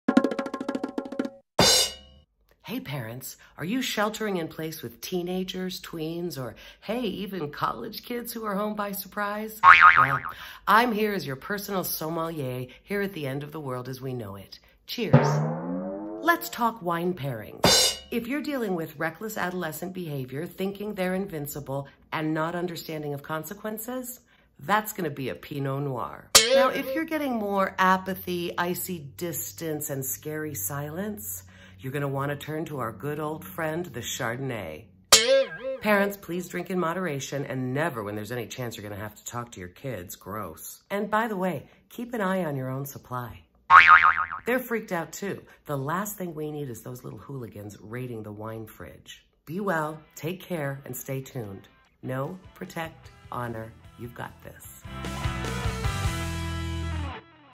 PRO TIPS FOR PARENTS: QUARANTINE EDITION (Wine Selections #1)
Lisa Lane Filholm writes and speaks about how to survive the balancing act of parenting adolescents.